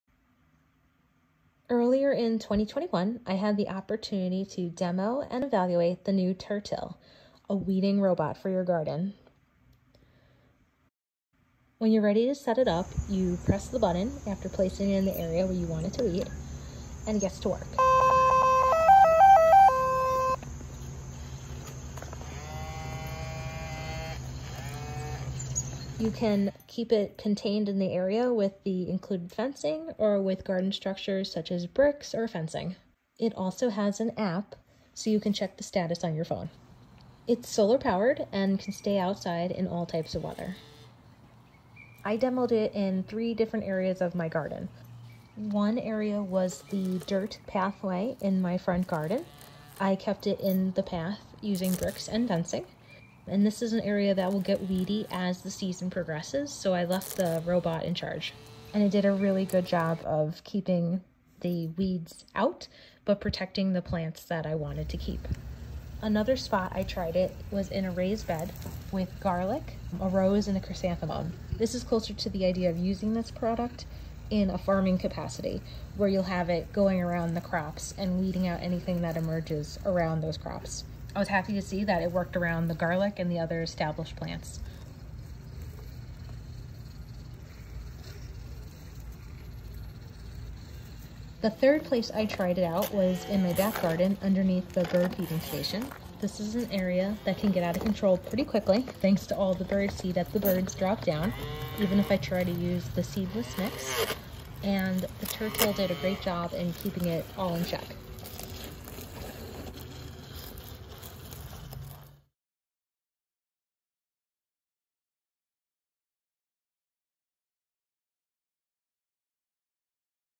Let this robot weed your garden: Tertill Review 2021
Set it, leave it, and walk away.

I was intrigued by the Tertill and its claim that it could remove weeds and not the plants I wanted to keep. I was sent a demo garden weeding robot to try out in my garden from April through September 2021. I tried it out in three different locations, and was pleased with the results.